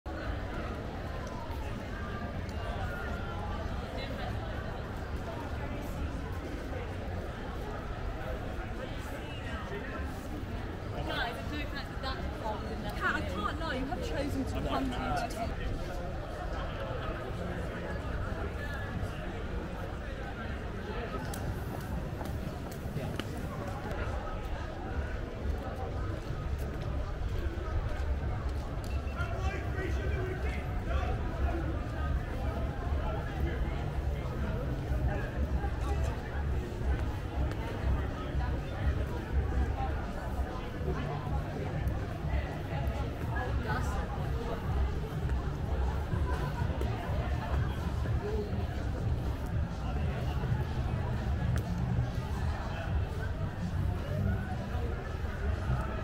The Mailbox & Cube Birmingham Nightlife
General views from Birmingham's Mailbox complex, including views of the canal and the Cube building.